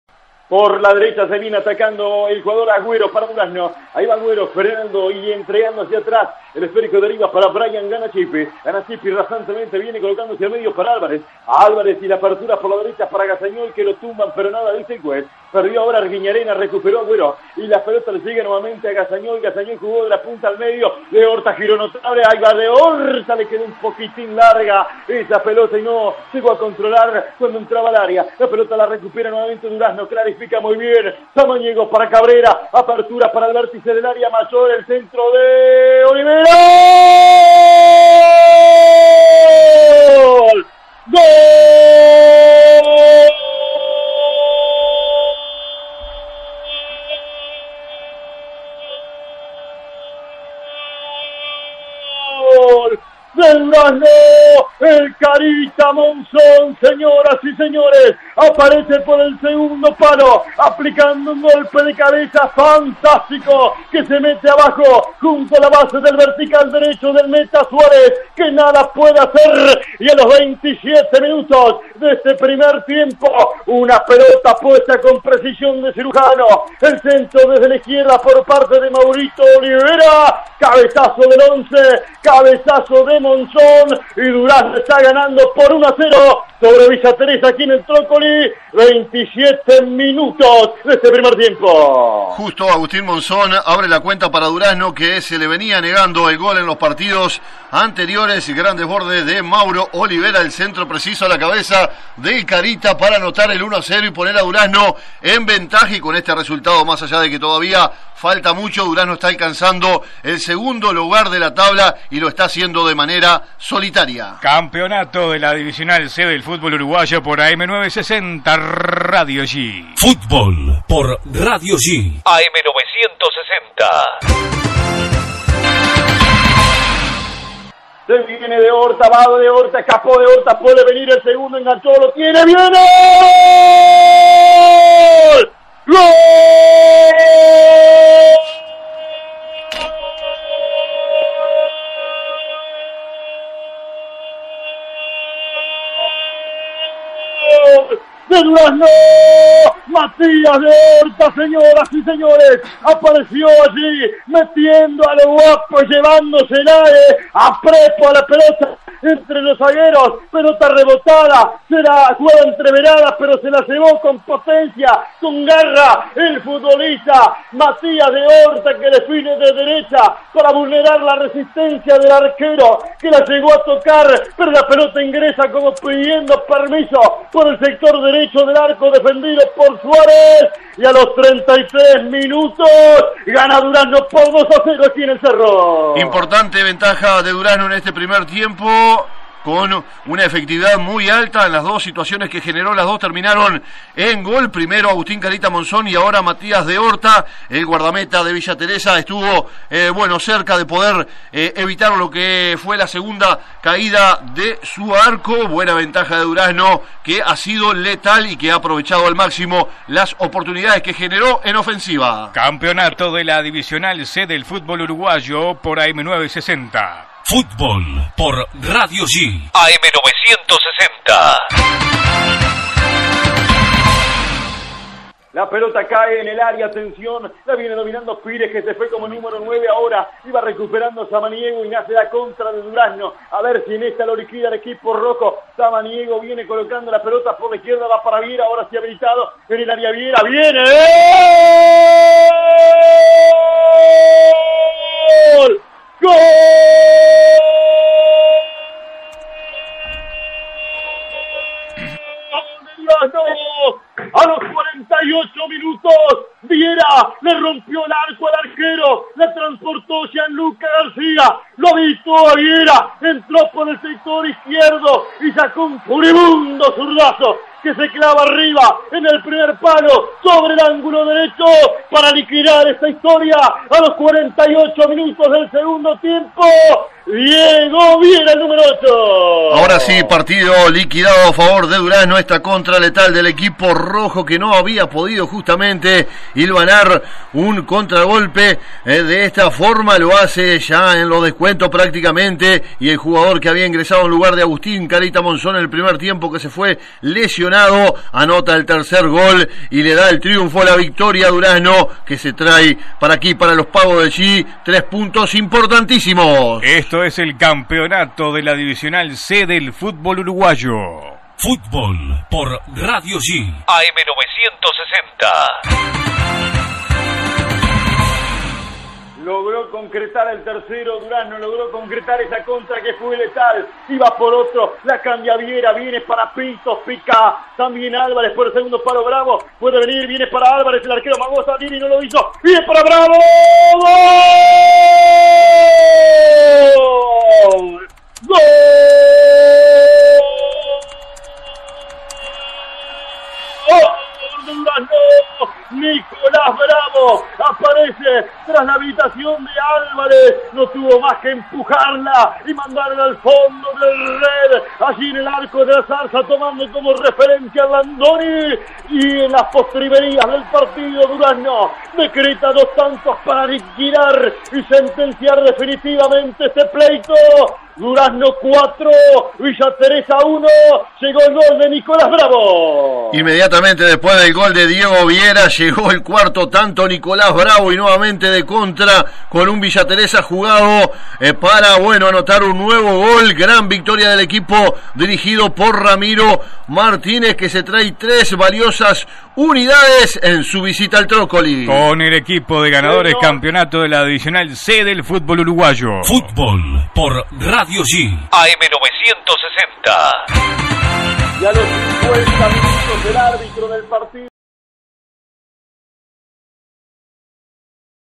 Villa Teresa 1 Durazno FC 4 - Primera C 2022 (por Brian Corbo)
Los goles de Durazno FC, por Brian Corbo para Radio Yí.